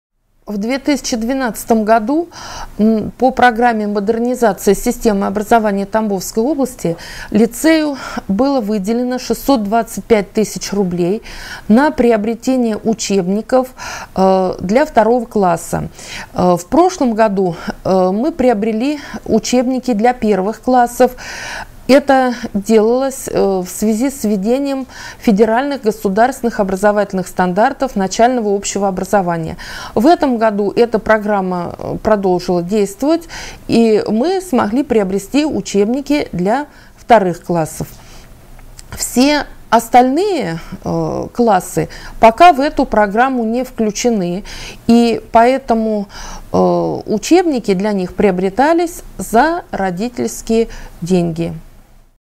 5 09 12 platnue uchebniki
На протяжении долгих  лет у родителей не болела голова — сколько средств из семейного бюджета нужно  потратить на покупку учебников для своего ребенка. В школьной библиотеке книги выдавали бесплатно. Но эта система умерла в конце девяностых. Обеспечение учебной литературой легло на плечи родителей. Недавно на центральных телеканалах прошла информация о том, что учебники для школьников вновь выдаются бесплатно. Как обстоят дела в нашем регионе, комментирует заместитель  директора Лицея.